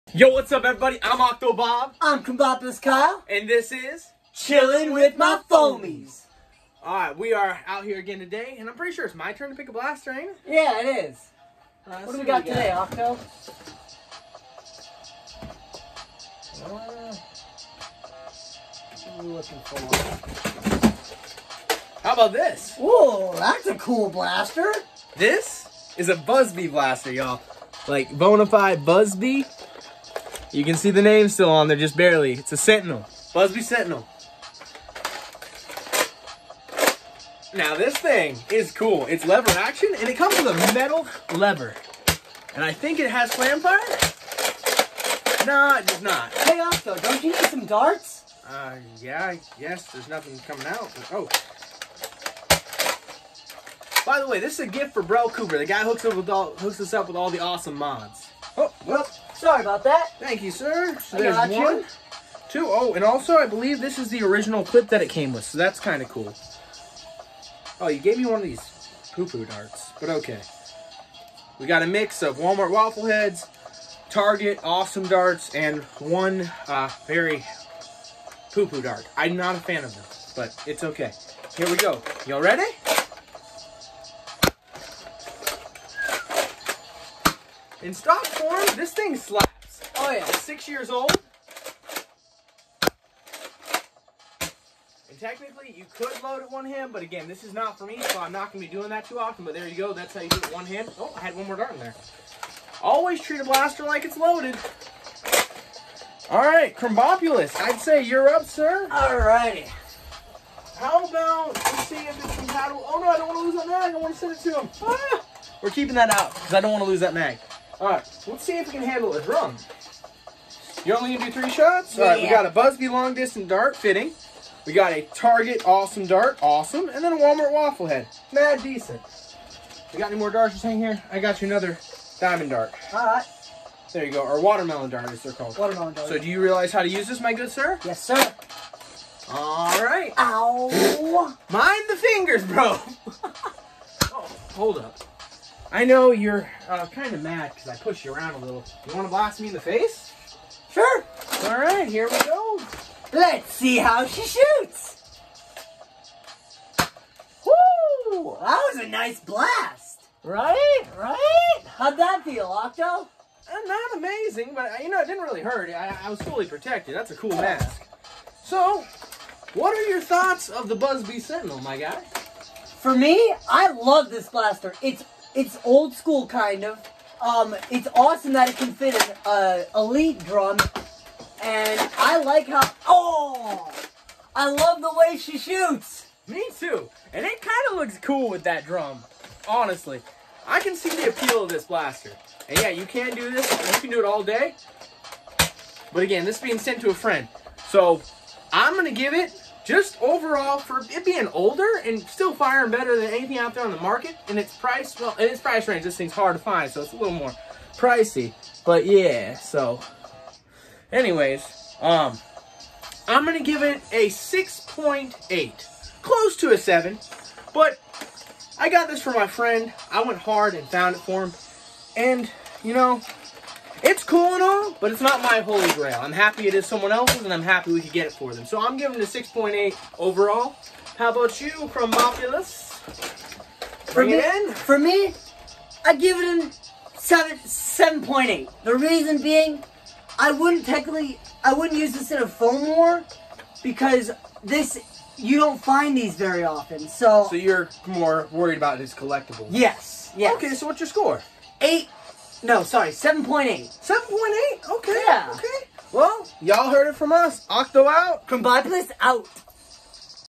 Random Blaster Reviews | Episode 37: BuzzBee Sentinel(OctoBob gets blasted in the face)🤣
Enter the BUZZBEE SENTINEL! Metal lever action,powerful,and highly collectible!
Our first BuzzBee brand blaster on the show. Krombopulos Kyle and OctoBob check out a blaster from the past that is still used and sought after in today’s modern foam flinging scene. Krombopulos Kyle gives OctoBob a dart that he doesn’t like,OctoBob smashes Kyles hand with the lever,and the buffoonery Is at an all time high🤣 DID WE MENTION OCTOBOB GETS BLASTED IN THE FACE?!🤪Thanks for watching, have a phenomenal day,and don’t forget to roast us In the comments 😛